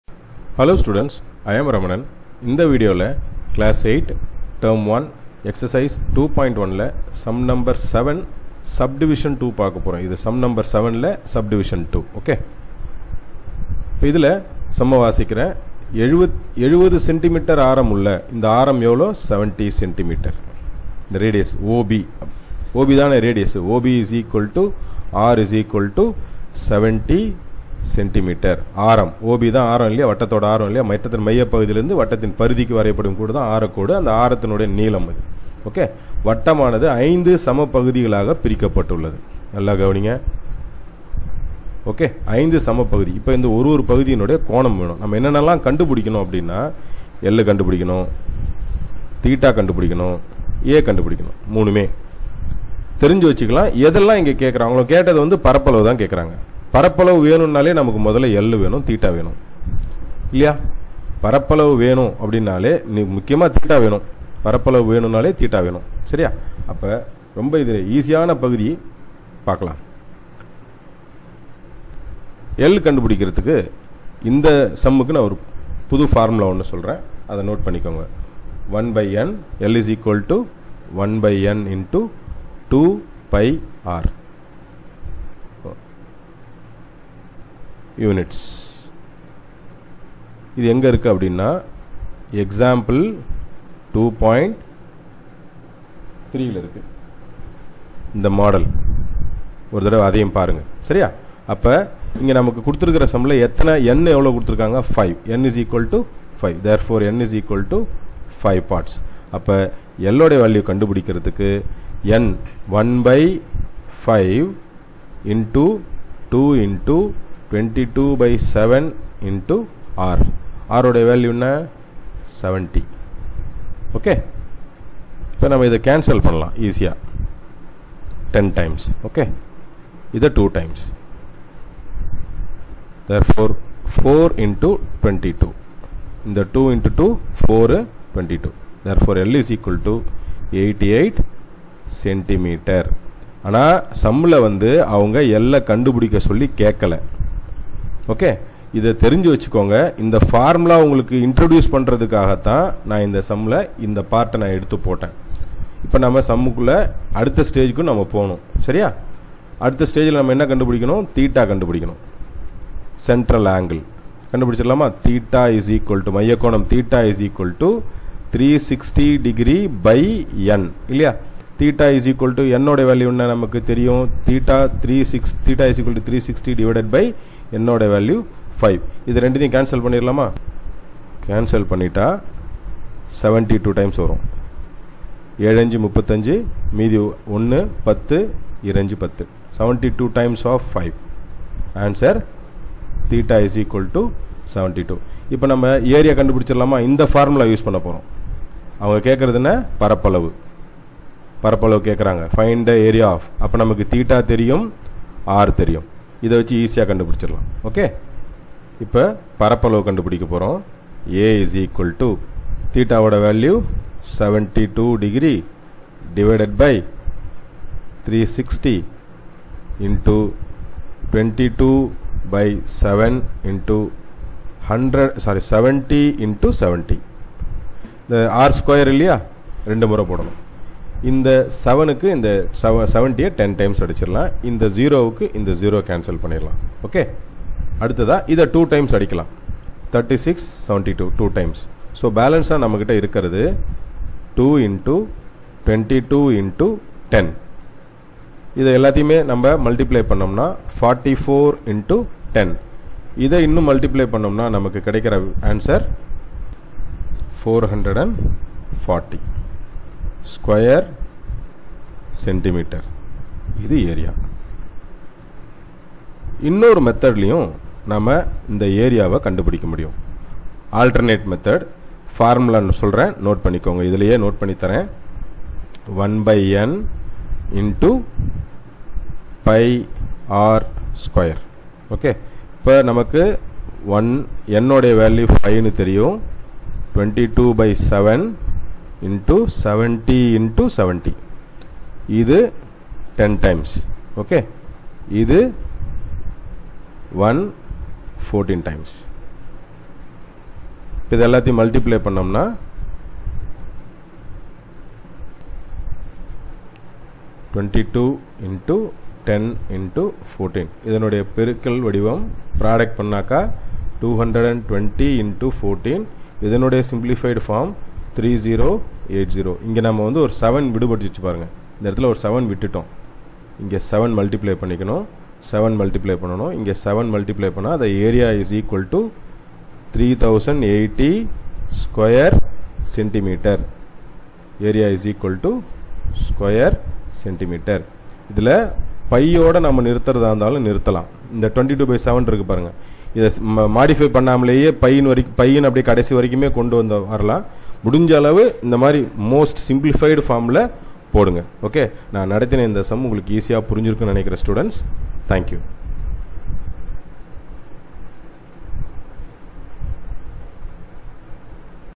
CLASS 8 - TERM I - EX 2.1 - SUM 7(ii)
இந்த காணொளியில் எட்டாம் வகுப்பு முதல் பருவத்திற்கு தேவையான பயிற்சி 2.1 இல்  7(ii) ஆவது கணக்கினை காணலாம்